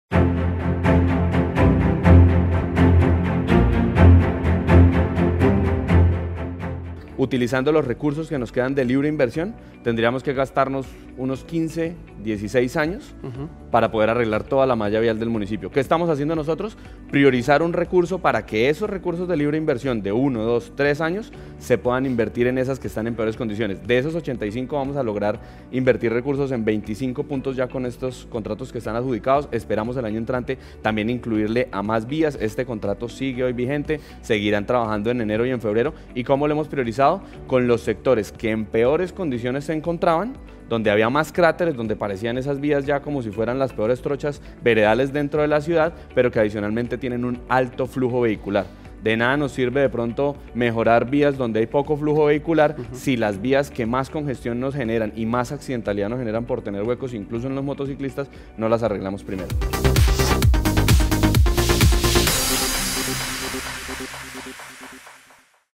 Miguel al Tablero - Rendición de Cuentas 2022 - Priorización de vías
MiguelAlTablero ✅ Priorizamos la recuperación de 25 puntos críticos para la movilidad en Floridablanca. 🛣️